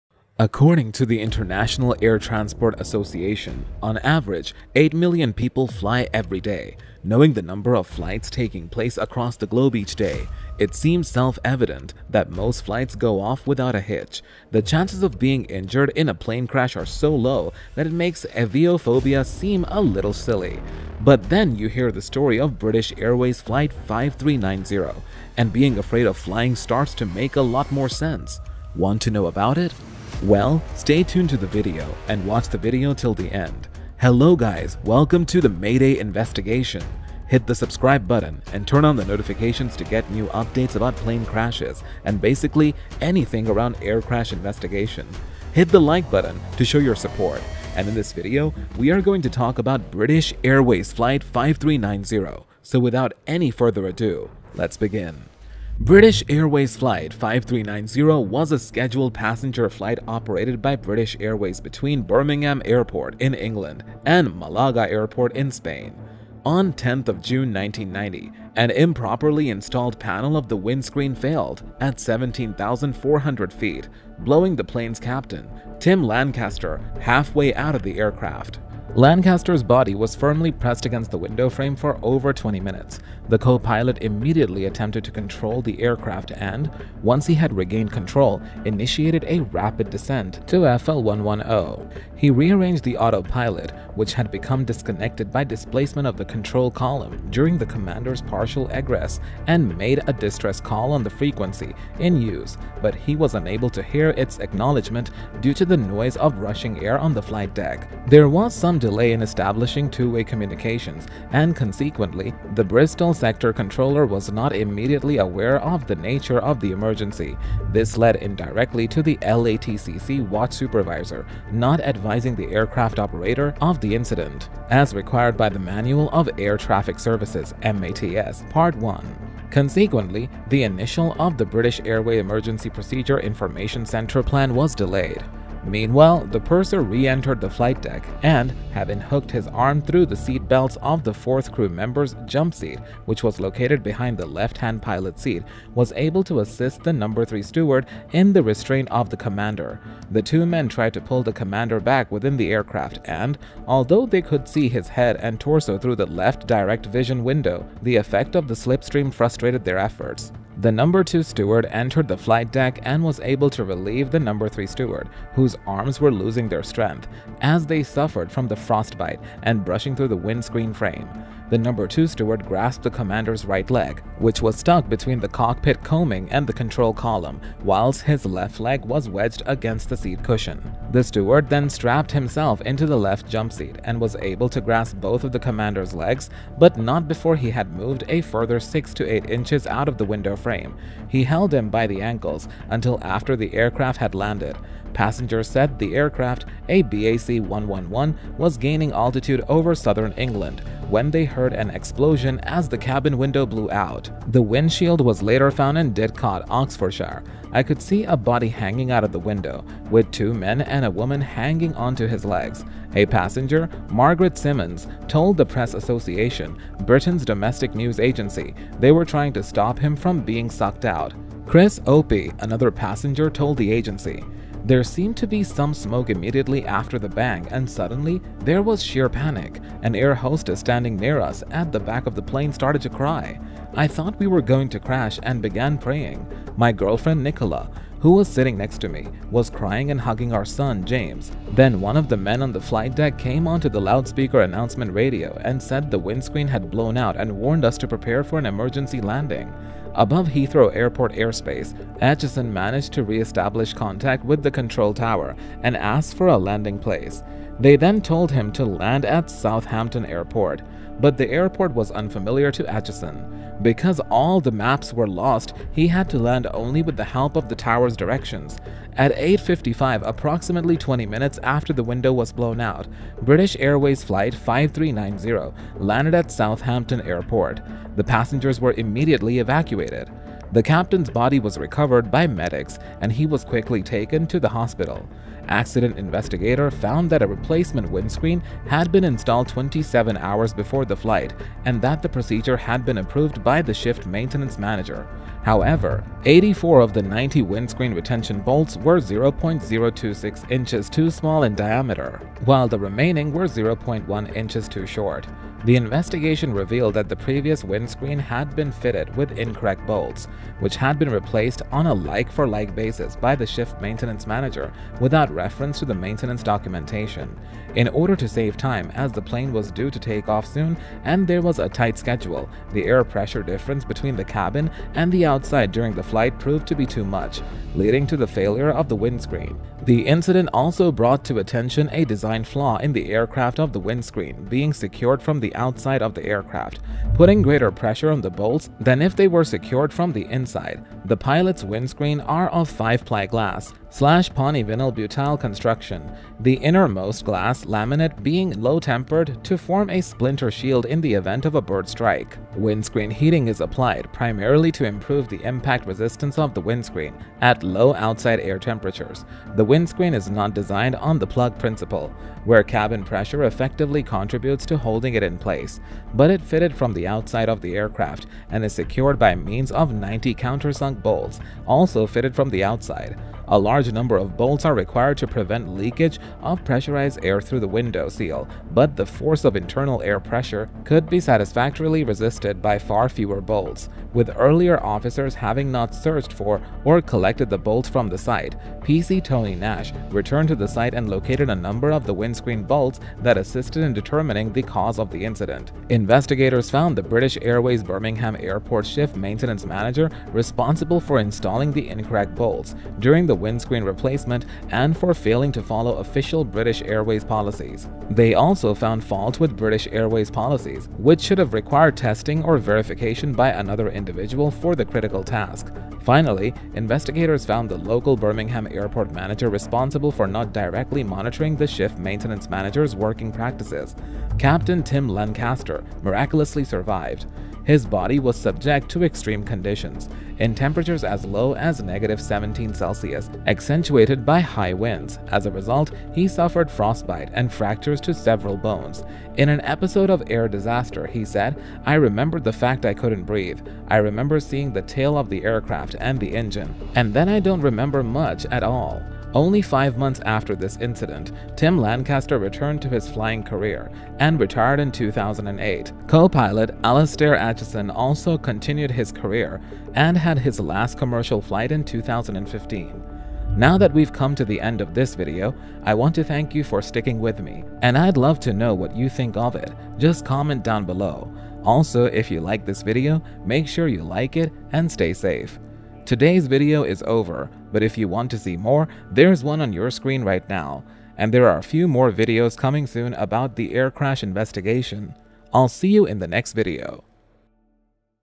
The Captain That Got Stuck Outside Of The Plane! | British Airways 5390
The Captain That Got Stuck Outside Of The Plane!

According to the International Air Transport Association, on average, 8 million people fly every day. Knowing the number of flights taking place across the globe each day, it seems self-evident that most flights go off without a hitch. The chances of being injured in a plane crash are so low that it makes aviophobia seem a little silly. But then you hear the story of British Airways Flight 5390, and being afraid of flying starts to make a lot more sense.

British Airways Flight 5390 was a scheduled passenger flight operated by British Airways between Birmingham Airport in England and Málaga Airport in Spain. On 10 June 1990 an improperly installed panel of the windscreen failed, at 17,400 feet (5,300 m), blowing the plane's Captain, Tim Lancaster, halfway out of the aircraft.

Lancaster's body was firmly pressed against the window frame for over twenty minutes. The co-pilot immediately attempted to control the aircraft and, once he had regained control, initiated a rapid descent to FL11O. He re-engaged the Autopilot which had become disconnected by displacement of the control column during the commander's partial egress and made a distress call on the frequency in use but he was unable to hear its acknowledgment due to the noise of rushing air on the flight deck.

There was some delay in establishing two-way communications and consequently, the Bristol Sector Controller was not immediately aware of the nature of the emergency. This led indirectly to the LATCC Watch Supervisor not advising the aircraft operator of the incident, as required by the Manual of Air Traffic Services (MATS) part 1. Consequently, the initiation of the British Airways Emergency Procedure Information Centre plan was delayed.

About Mayday Investigation:
⚙️ Produced, voiced and written by: Mayday Investigation!
🎬 Videos about aviation technology, air accidents and incidents!